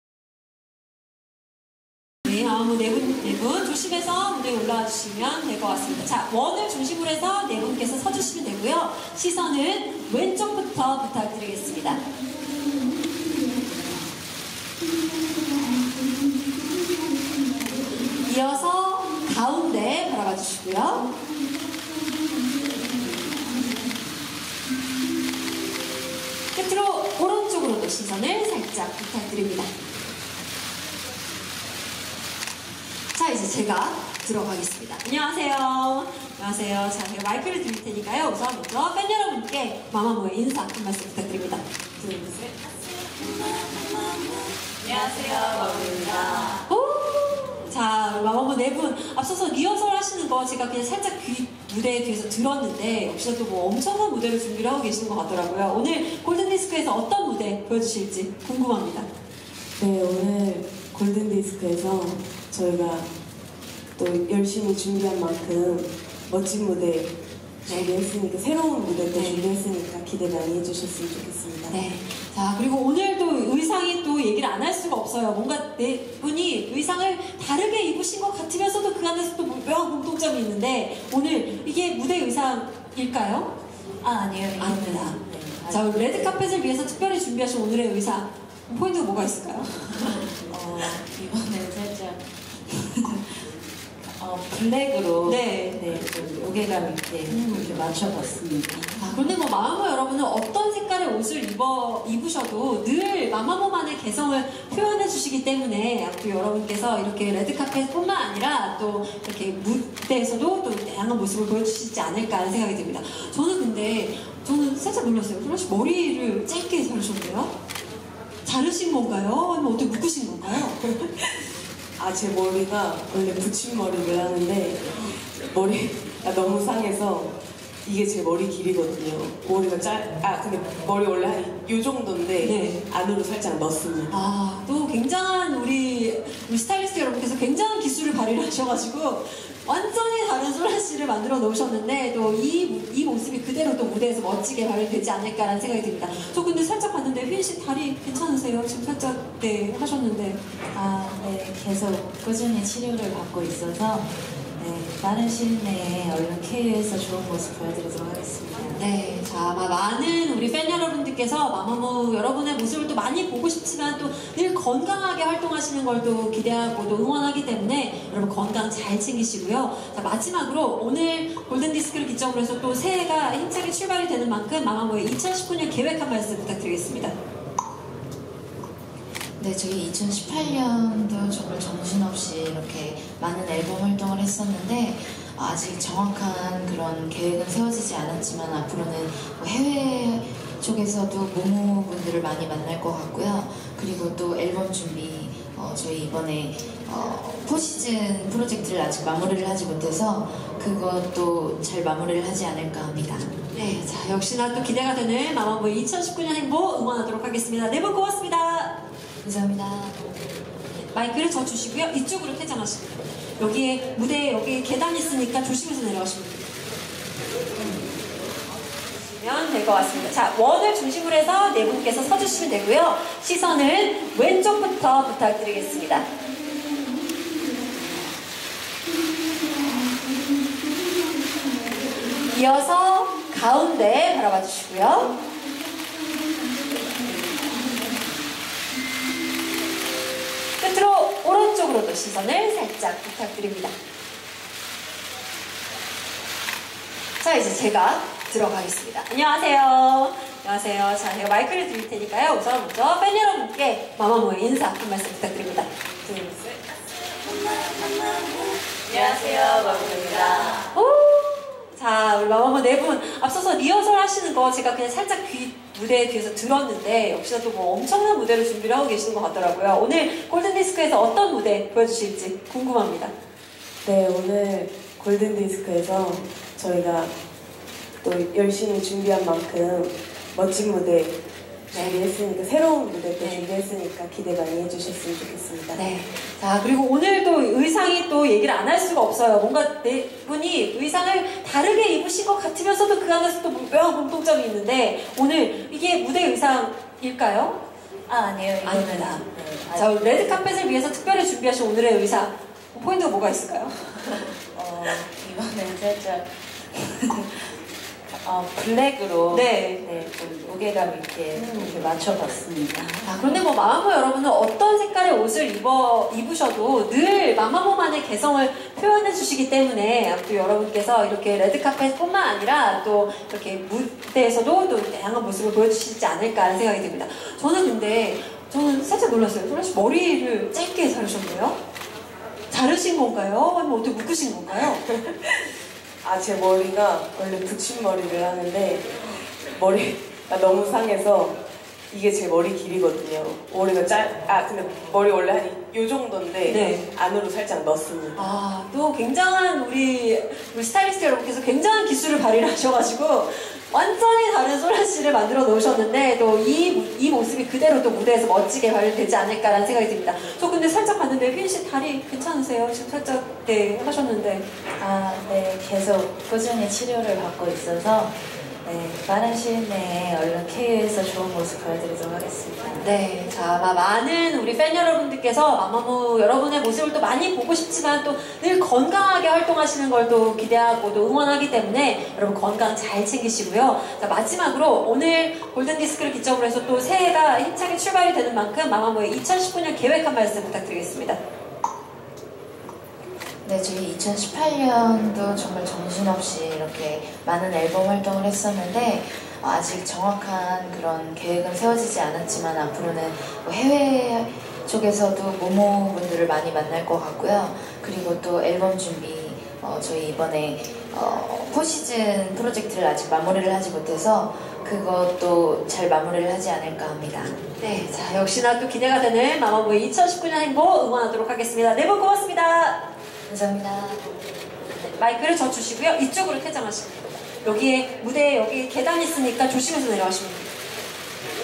0105 마마무(MAMAMOO) 솔라X휘인 포커스 33회 골든디스크 시상식 4K 영상
1월5일 오후 서울시 고척동 고척스카이돔에서 '제33회 골든디스크시상식' 디지털음원부문 시상식 레드카펫 행사가 열려 포토월에서 마마무가 포즈를 취하고 있다.